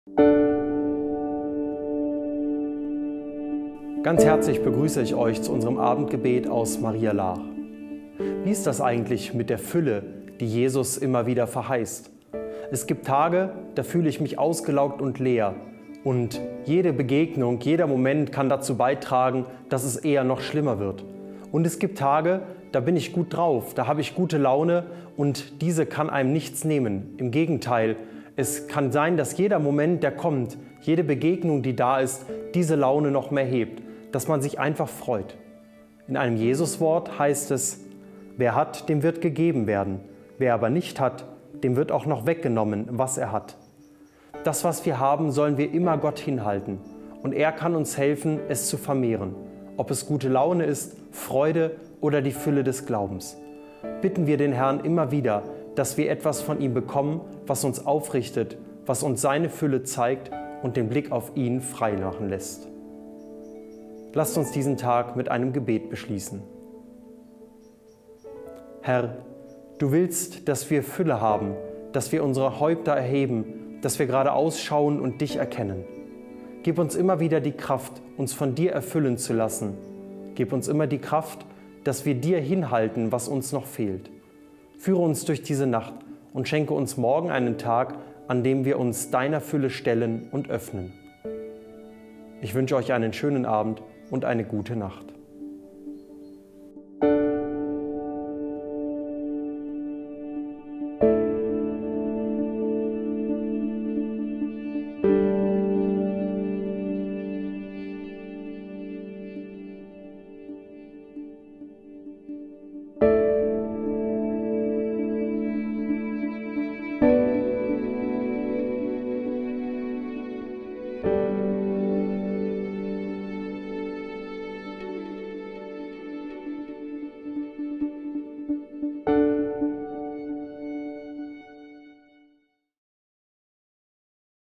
Abendgebet aus Maria Laach 08. Februar 2021
Manchmal fällt dem ausgelaugte Pater Philipp alles schwer. An anderen Tagen aber trägt ihn die gute Laune mit Leichtigkeit von Aufgabe zu Aufgabe. Jesus greift diese Erfahrung in einem berühmt gewordenen Wort auf (vgl. Mt 25,29).